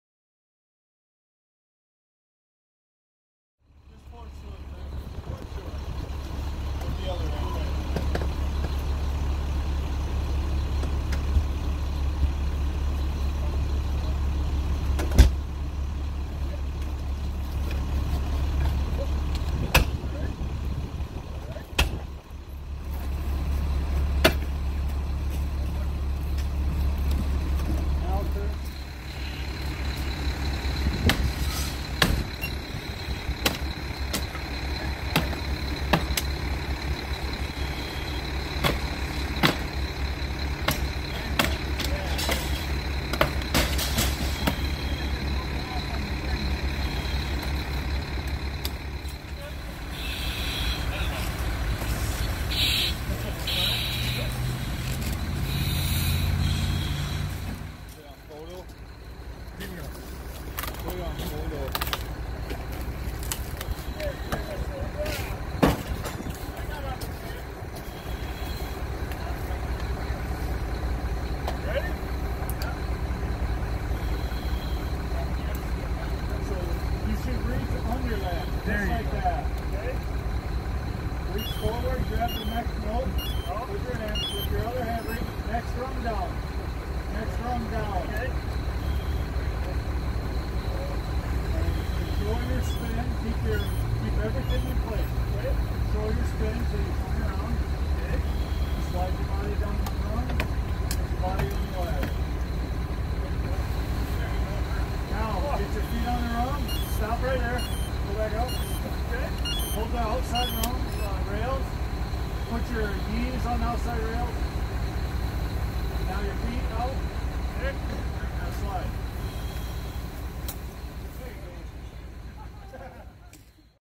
NEWS ADVOCATE: Manistee Fire Department training day, Feb. 24, 2022
The house on 269 Fifth St. was the site of training for the Manistee City Fire Department on Thursday. Shown is Brandon Nelson using a maneuver to go head first out a window and then turn facing the ladder and slide down the length of the ladder quickly.